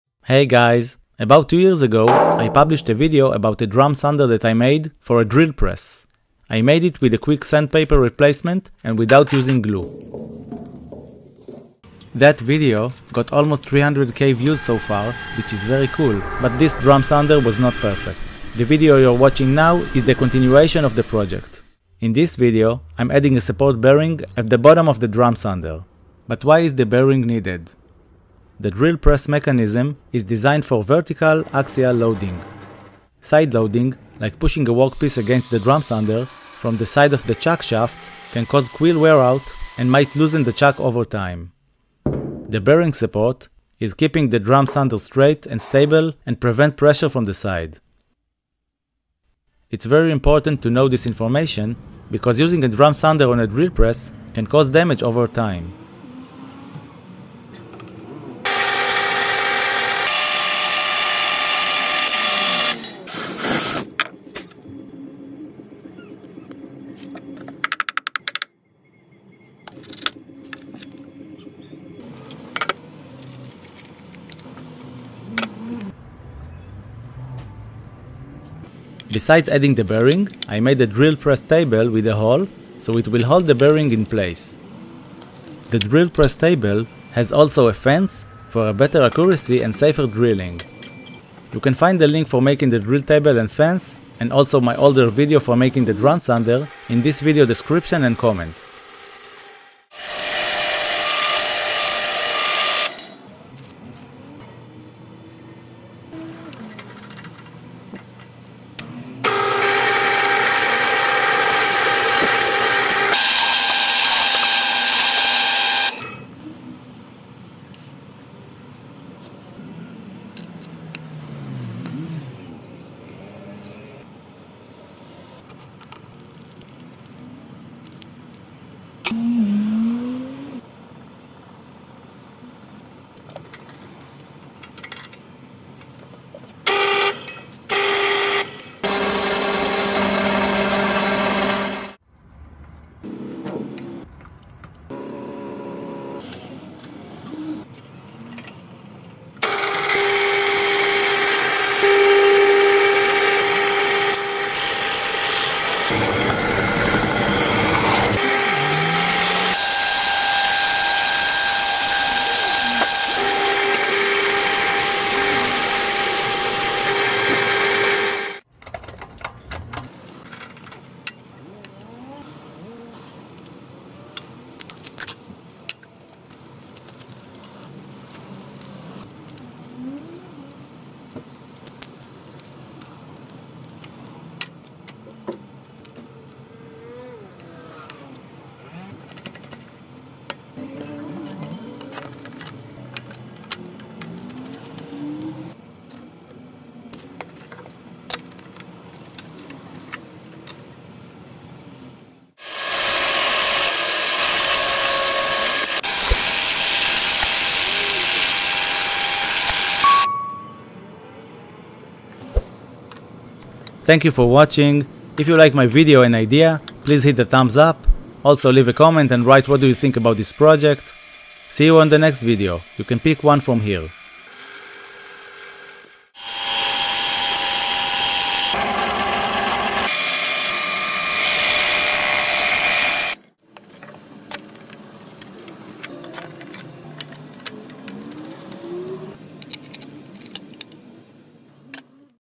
I Added a Bearing to My Drum Sander for Drill Press. Why? | XDIY
It's a MUST! I Improved my Drum Sander for the drill press and added A Bearing...

About 2 years ago, I published a video about a DIY drum sander that I made for drill press, with a quick sandpaper replacement and without using glue, but this drum sander wasn’t perfect.

The video you’re watching now, is the continuation of the project.
ln this video, I’m adding a support bearing at the bottom of the drum sander.
Why is it needed?
It’s important to know this information, because using a drum sander on a drill press can cause damage over time.
The drill press mechanism is designed for vertical axial loading. Side loading, like pushing a work piece against the drum sander, from the side of the chuck shaft, can cause quill wear out and might loosen the chuck over time.
The bearing support helps keeping the drum sander straight and stable and prevent pressure from the side.

Besides adding the bearing, I made a drill press table with a hole, so it will hold the bearing in place. The drill press table has also a fence for better accuracy and safer drilling.

Thanks!

תוף ליטוש למקדחת עמוד - עשה זאת בעצמך